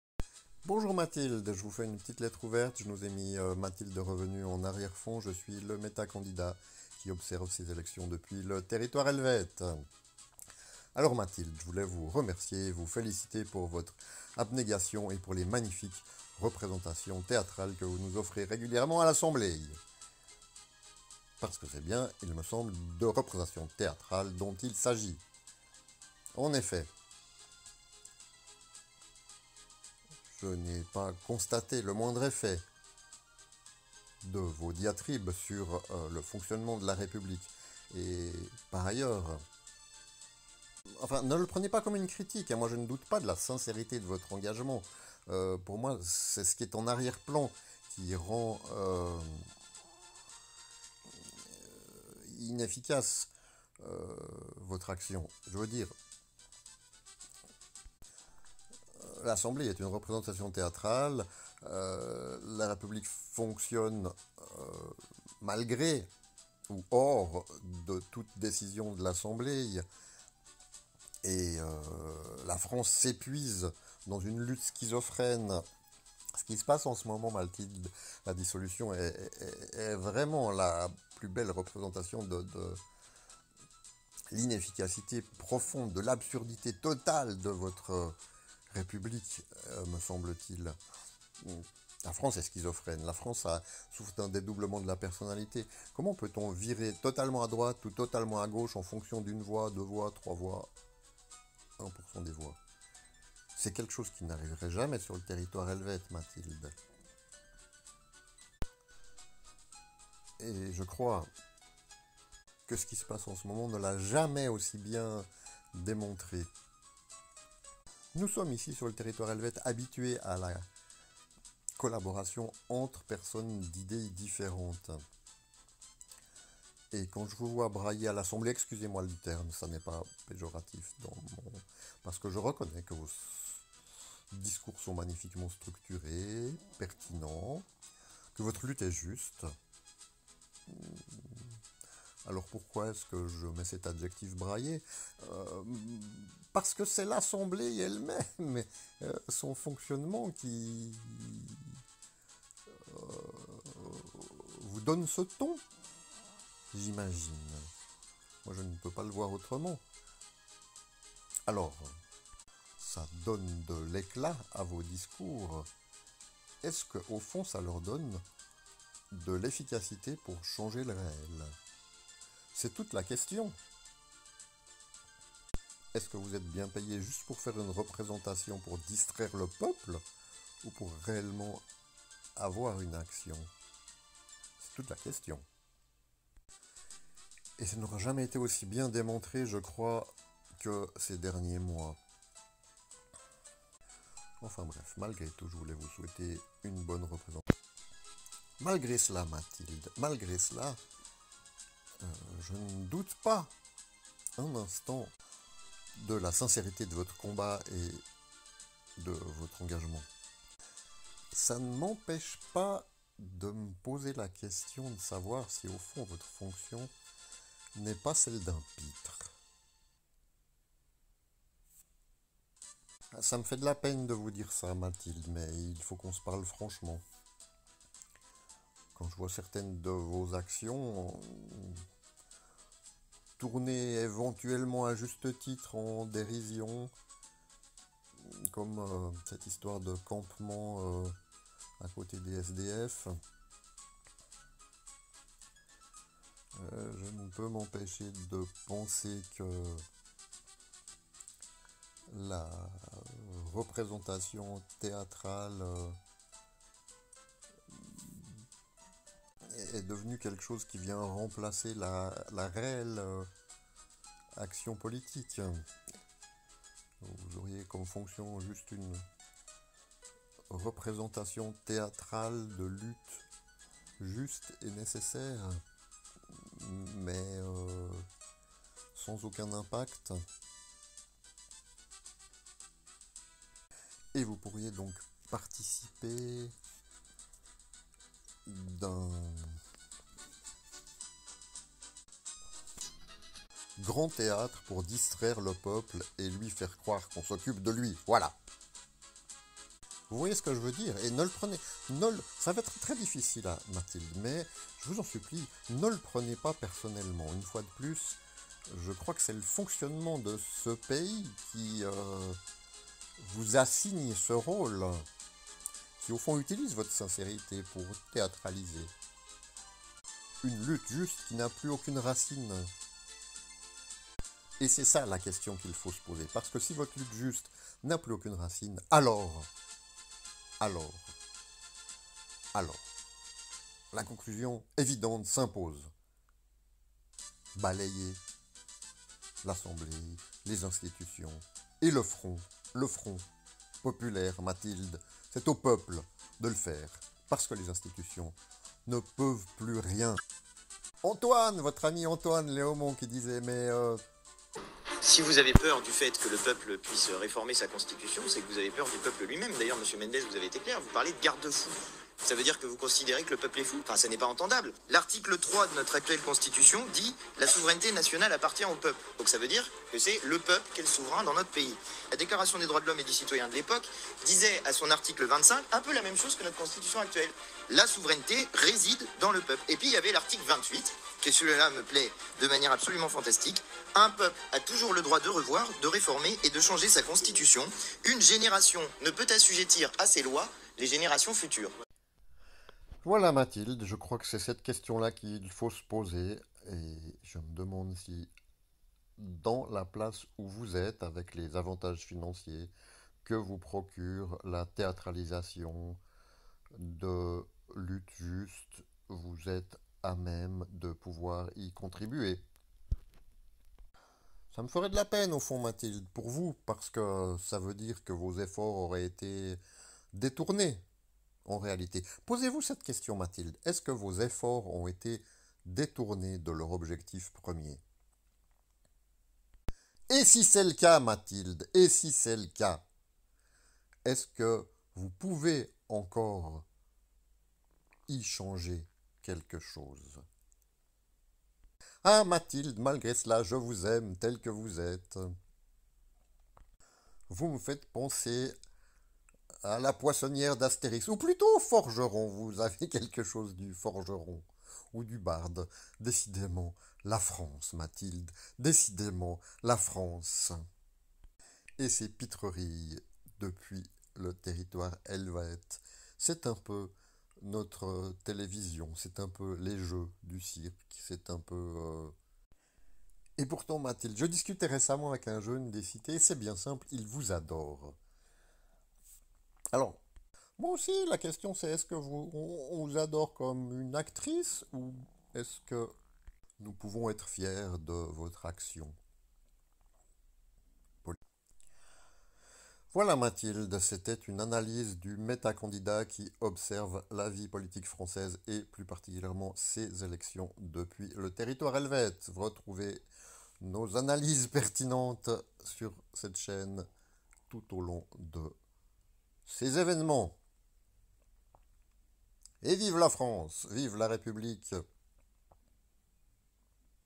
lettre ouverte à Mathilde Pannot
en tant que méta candidat j'observe et je commente ces élections